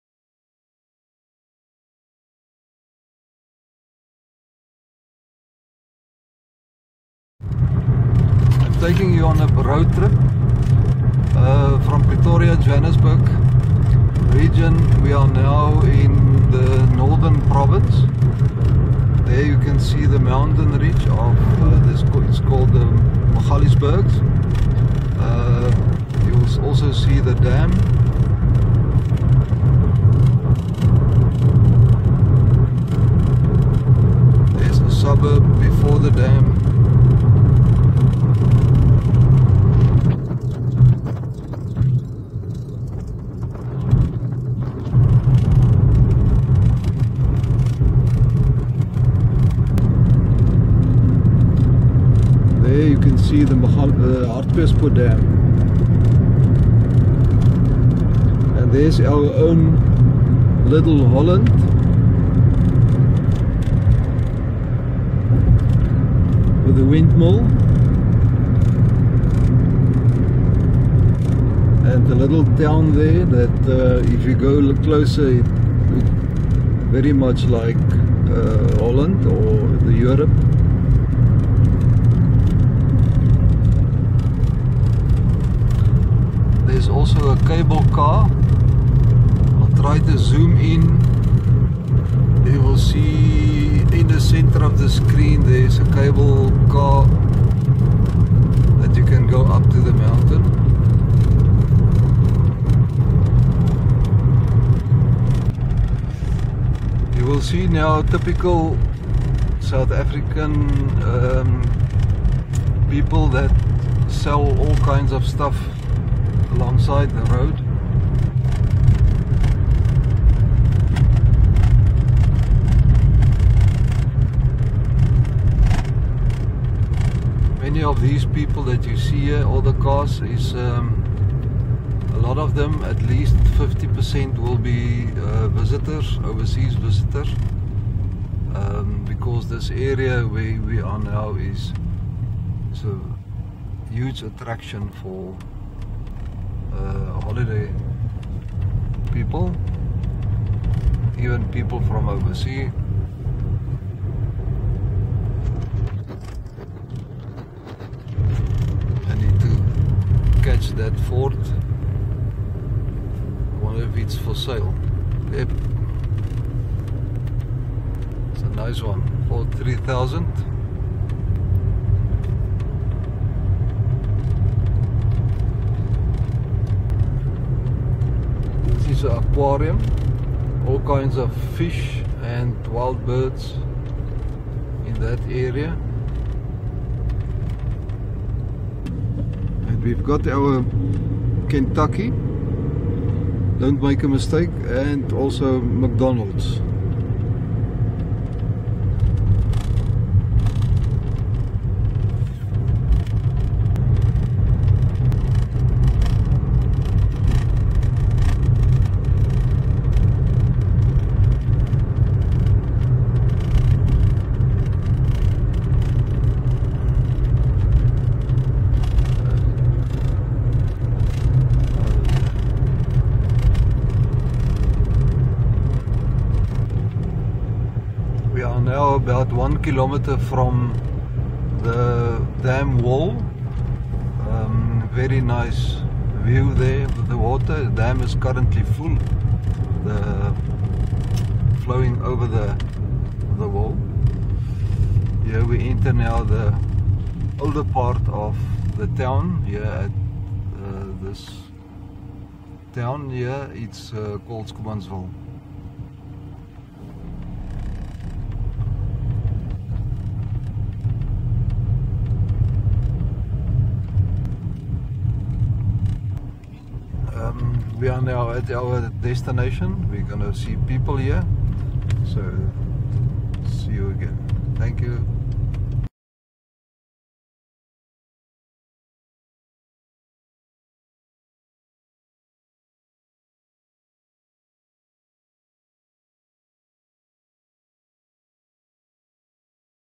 Road trip to Hartbeespoort Dam in South Africa North West Province
This is a video of the beautiful scenery at the Hartbeespoort Dam in South Africa